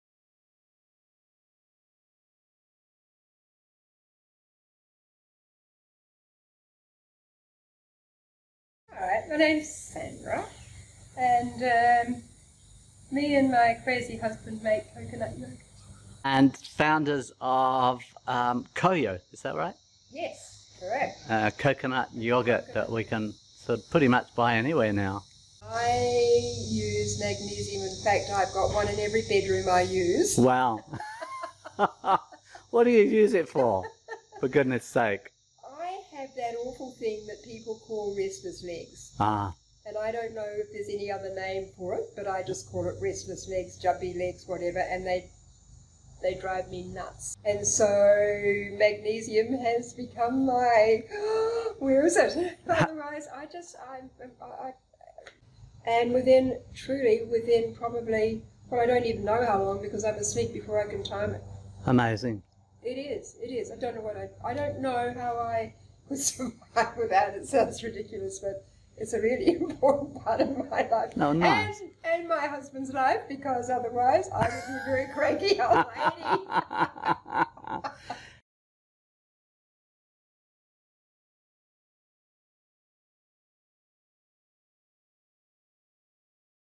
COYO CO-Founder talks about Amazing oils Sleep Lotion
Sandra talks about Restless Legs and Amazing Oils new Sleep Lotion.